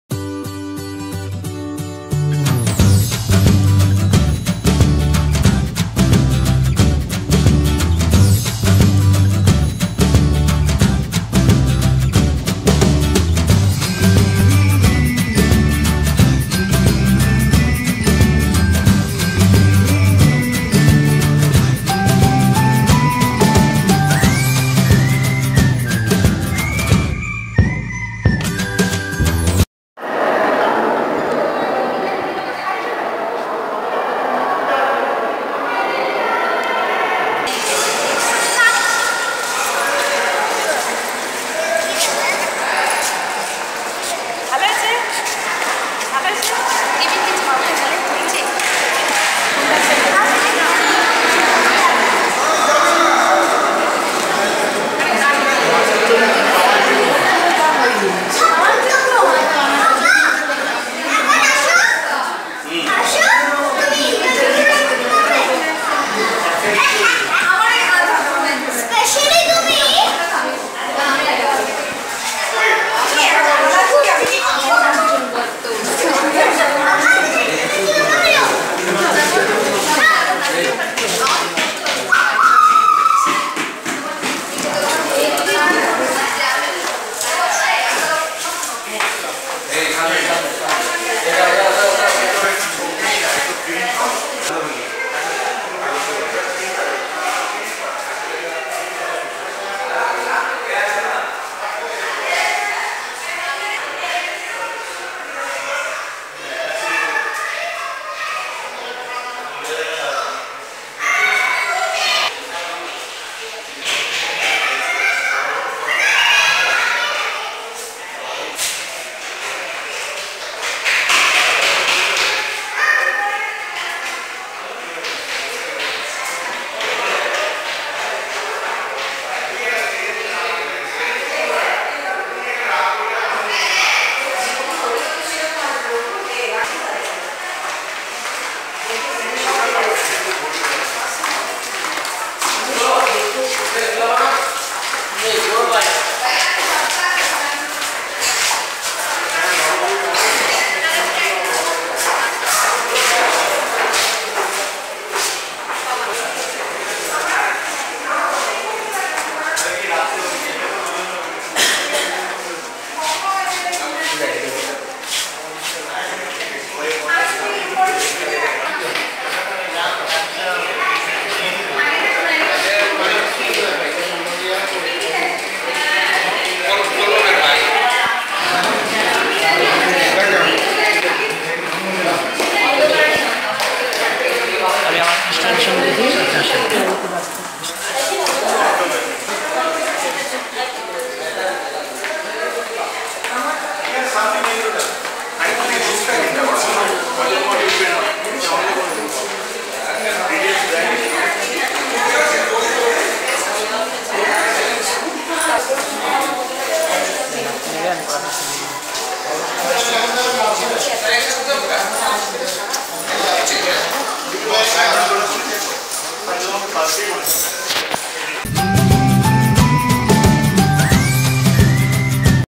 International Exhibition & Competition for children and Young adults Bengal Gallery - ICCR
4th International Exhibition & Competition for children and Young adults
At Bengal Gallery, ICCR Kolkata

Bengal Gallery - ICCR Kolkata

This is a Tips & Tutorial channel.